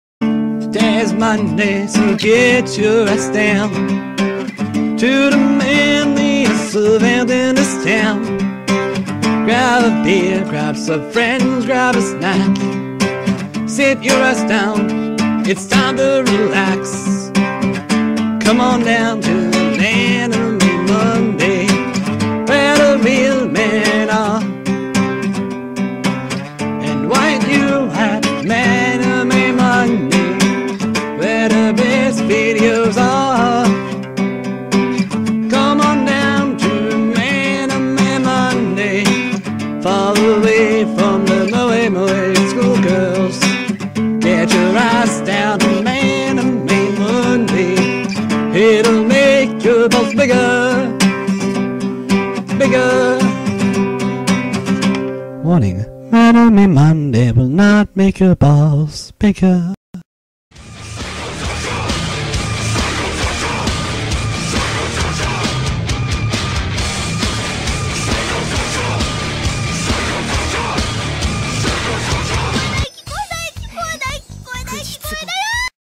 Manime Monday Add
An add I made for manime monday using a song I made.